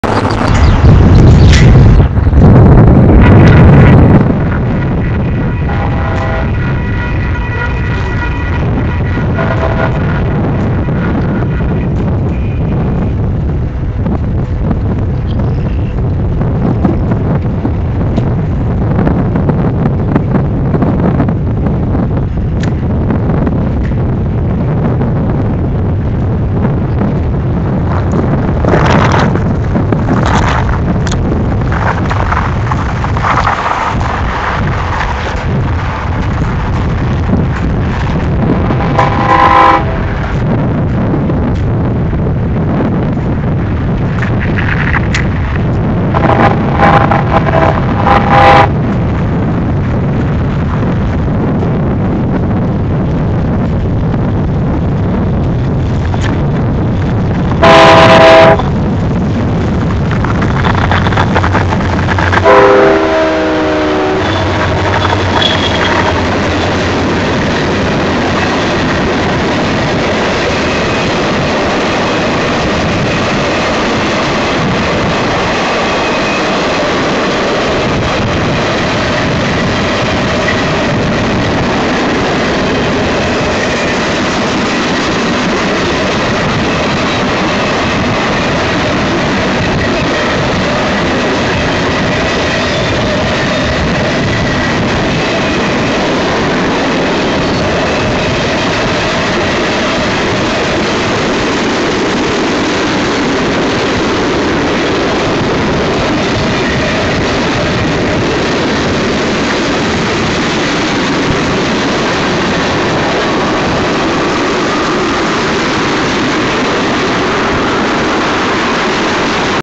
WB UPRR Manifest Train Navigating the UPRR Triple Track Mainline @ Gibbon Jct NE.avi
WB UPRR manifest train navigating the UPRR Triple Track Mainline @ Gibbon Jct NE while traveling towards N. Platte NE

This video was shot in May 2005